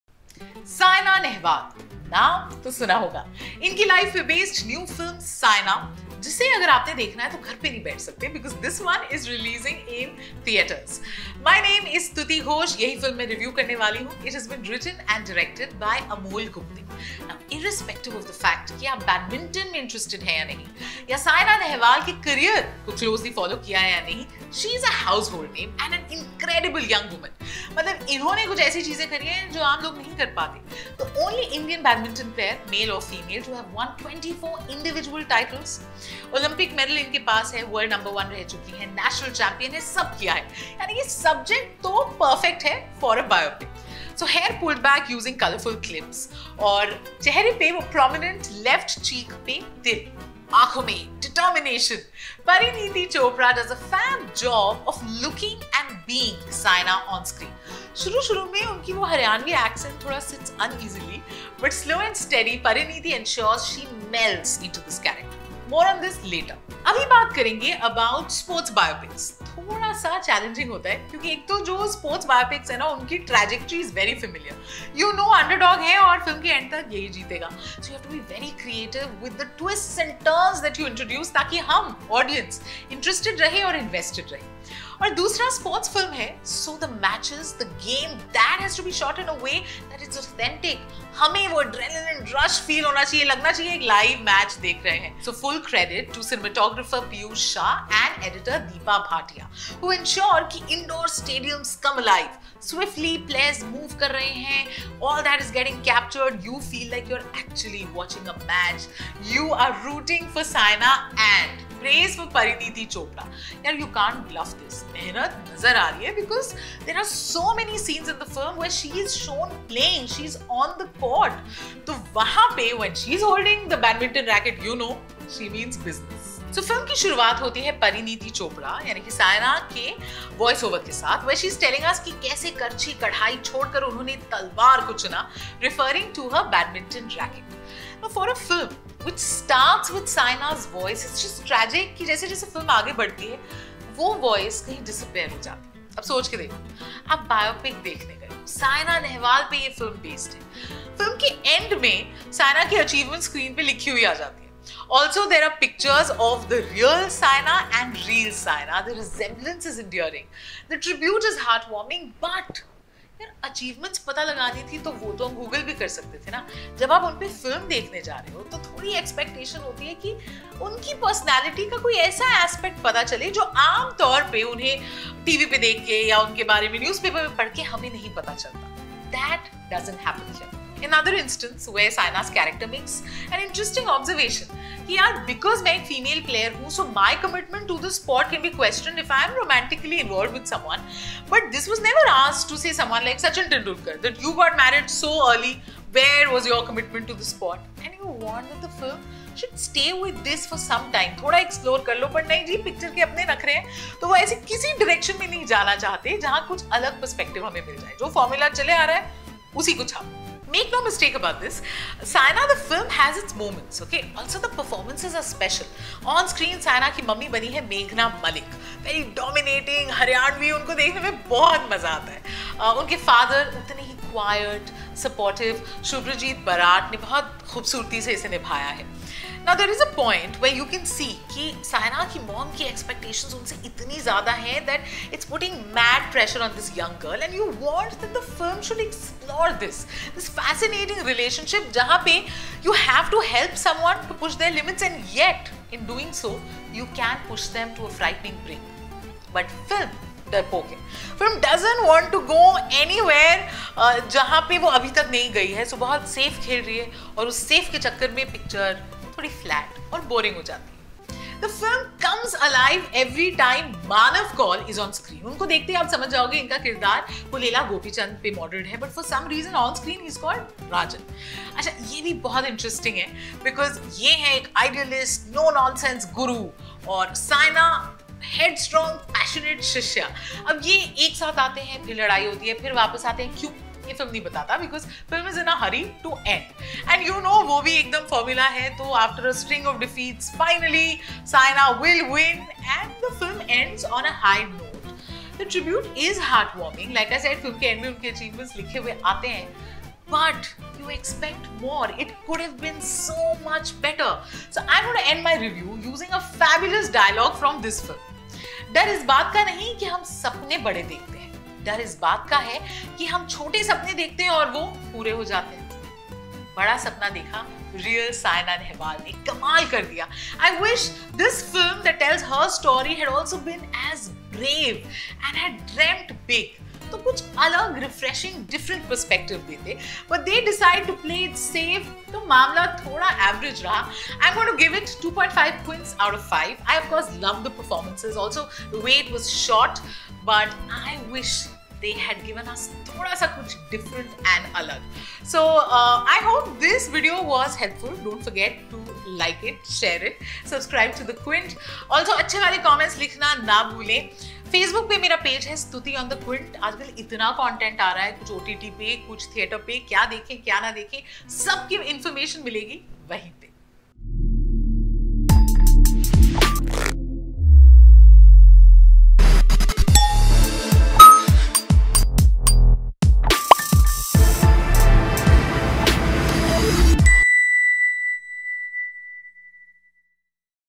Saina Film Review | Despite Its Moments, 'Saina' Fails To Think Out of the Box | The Quint
Rj Stutee's movie review of Parineeti Chopra starrer Saina

Quick Guide:
0:00 Saina Nehwal, naam toh suna hoga
0:44 Parineeti as 'Saina'
1:07 Challenges with sports biopics
1:35 Hard work done to bring authenticity
2:10 What's missing from the movie?
3:30 Performances that made the movie special
4:05 Why 'Saina' doesn't live up to expectations